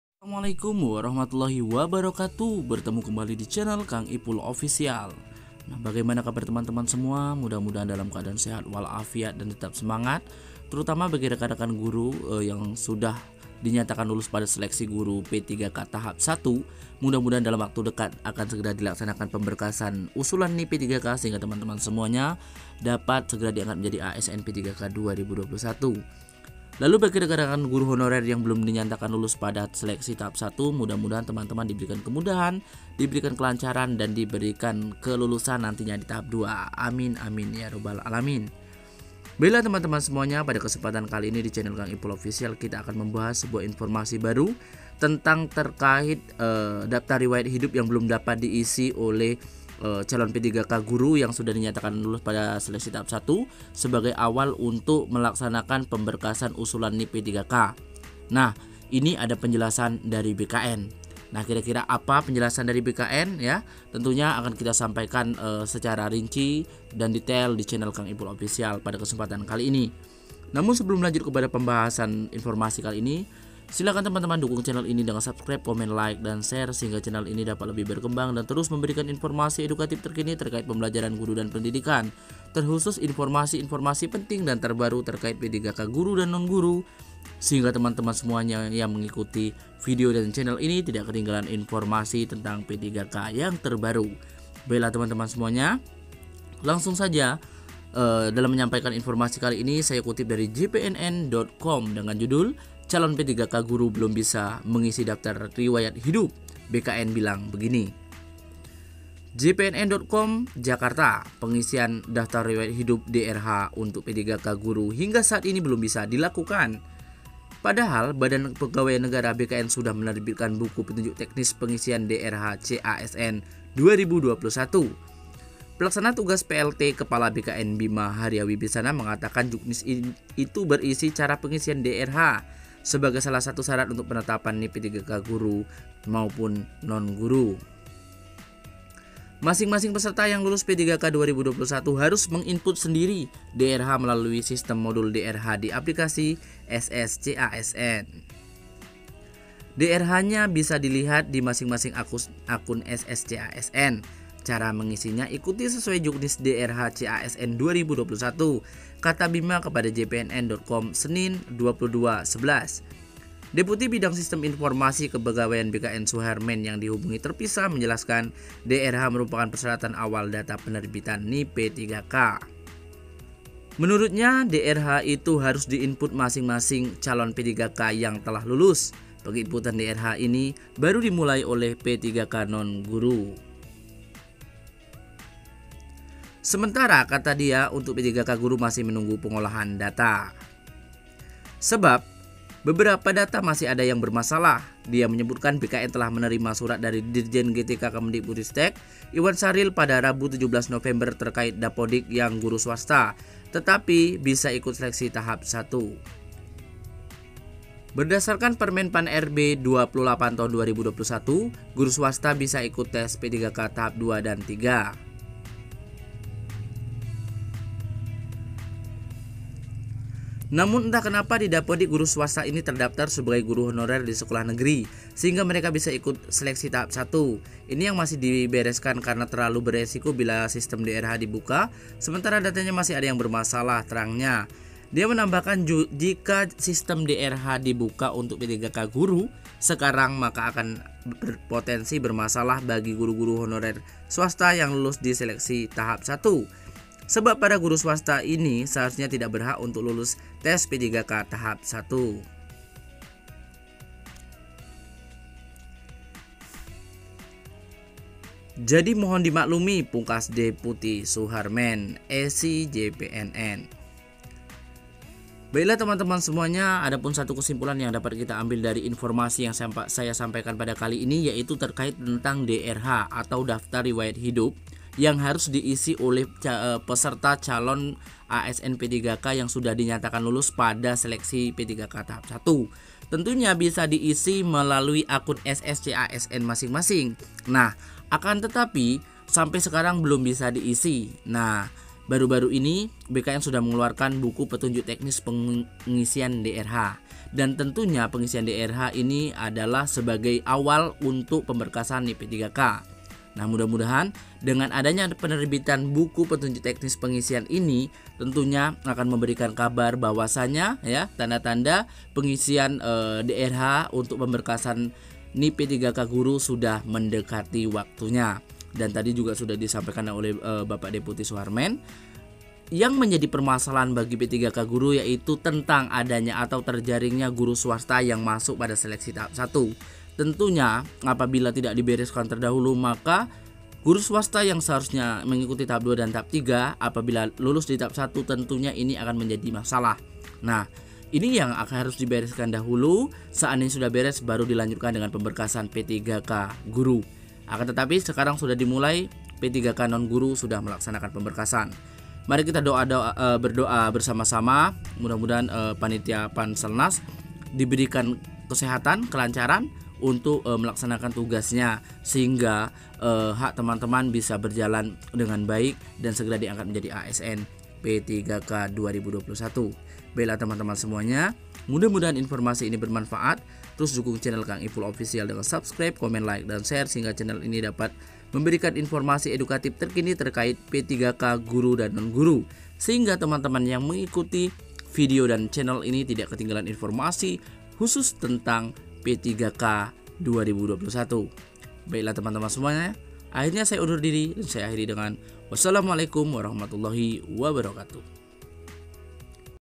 DRH PPPK Guru Belum dapat diisi. Simak Penjelasan Kepala BKN dan Deputi Suharmen
Daftar riwayat hidup merupakan awal untuk di laksanakan pemberkasan PPPK Guru Tahap 1. Akan tetapi sampai saat ini belum dapat diisi. Semoga dalam waktu dekat Guru Lulus Tahap 1 sudah bisa mengisinya.